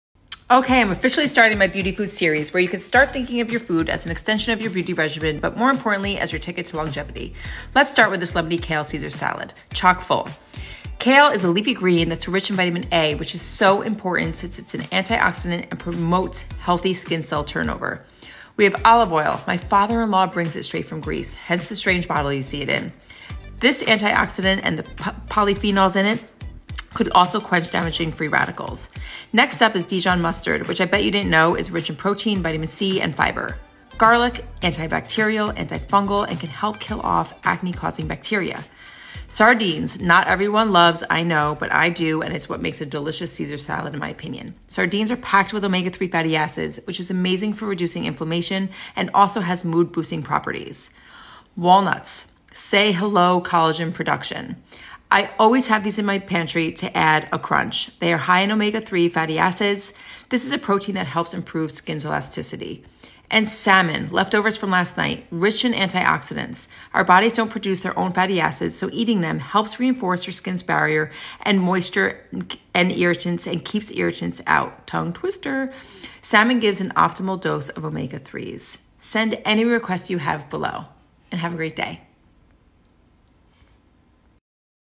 Delicious and Nutritious: Lemony Kale Caesar Salad for Beauty Inside Out
I’m launching my Beauty Foods series with this easy Lemony Kale Caesar Salad. Elevate your salad game and treat yourself to a beauty-boosting meal.